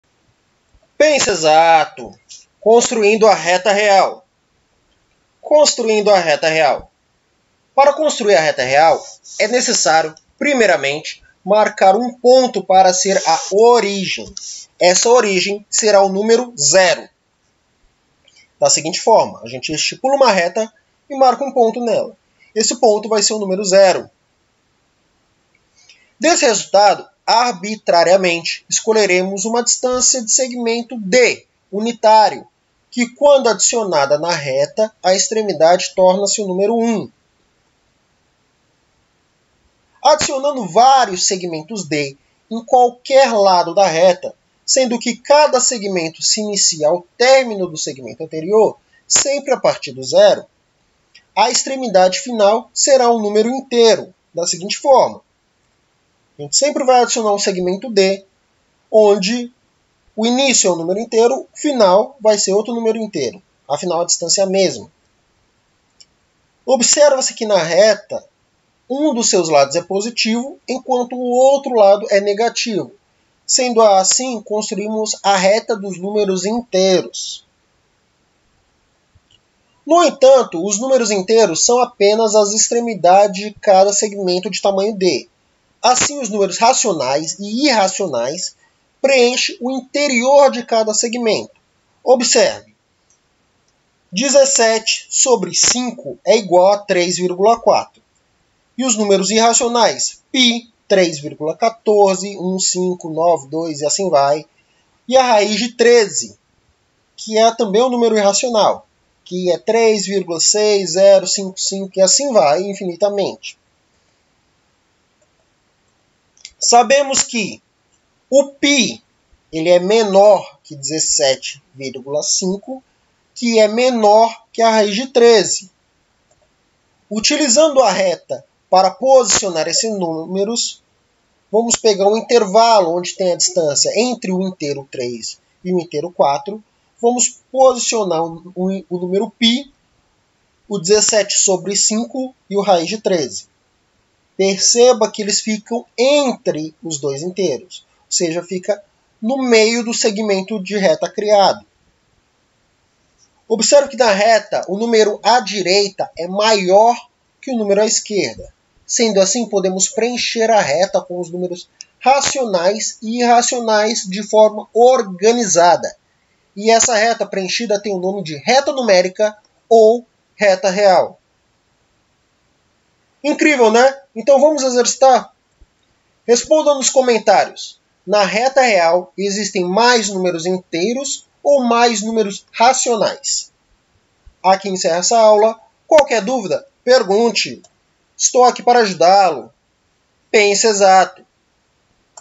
CONSTRUINDO A RETA REAL | MATEMÁTICA BÁSICA
APRENDA A CRIAR UMA RETA REAL / RETA NUMÉRICA

CURSO DE MATEMÁTICA BÁSICA